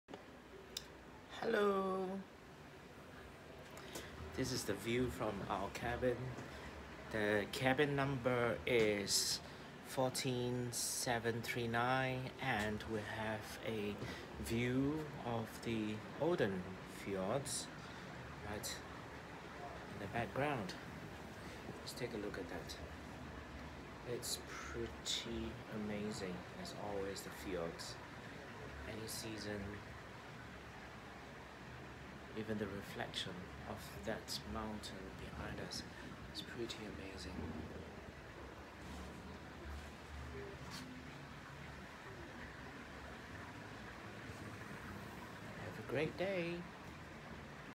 Olden (Norway) Views from Balcony cabin 14739 P&O Cruises Iona
Lovely views of the stunning glacial landscape!